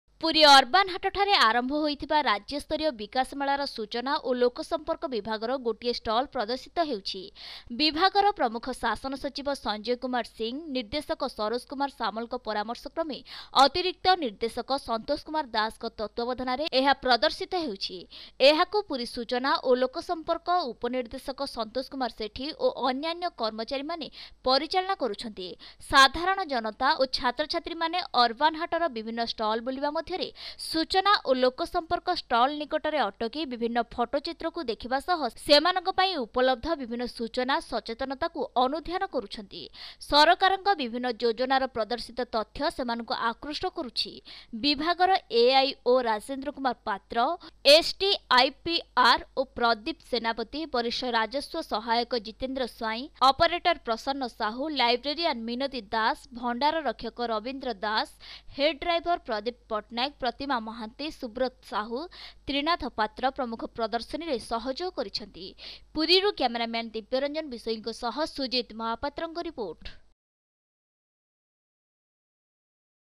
ରାଜ୍ୟ ସ୍ତରୀୟ ବିକାଶ ମେଳାରେ ସୂଚନା ଓ ଲୋକ ସମ୍ପର୍କ ବିଭାଗର ଷ୍ଟଲ ପ୍ରଦର୍ଶିତ II State level development fair
A stall of the Department of Information and Public Relations is being displayed at the state level development fair which has started at Puri Urban Hat. It is being displayed under the supervision of Additional Director Satish Kumar Das in consultation with the Principal Secretary Sanjay Kumar Singh, Director Saroj Kumar Samal.